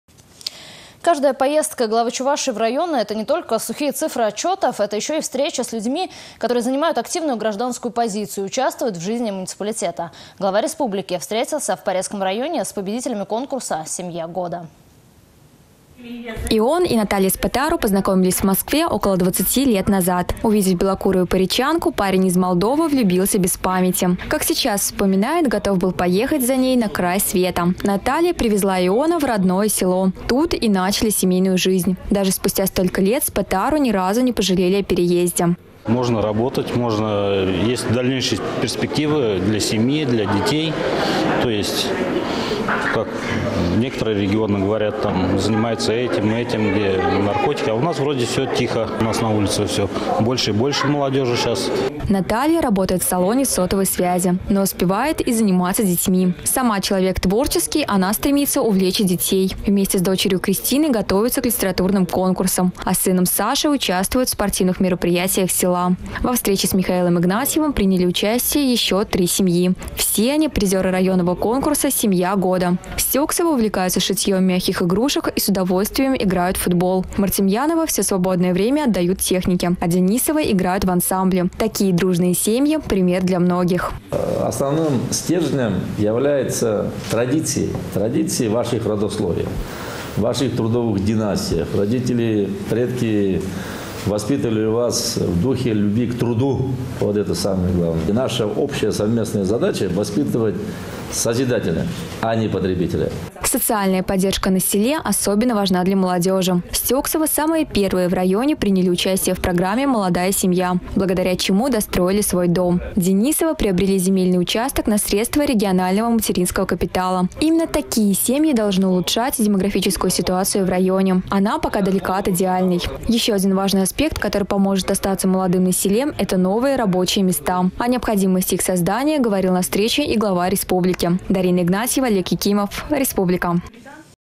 Глава Чувашии Михаил Игнатьев встретился семьей из с. Порецкое
Каждая поездка Главы Чувашии  в районы — это не только  сухие цифры отчетов, это еще и встречи с людьми, занимающими активную гражданскую позицию, участвующими  в жизни  муниципалитета.  Глава  республики  встретился в  Порецком районе с  победителями конкурса «Семья года».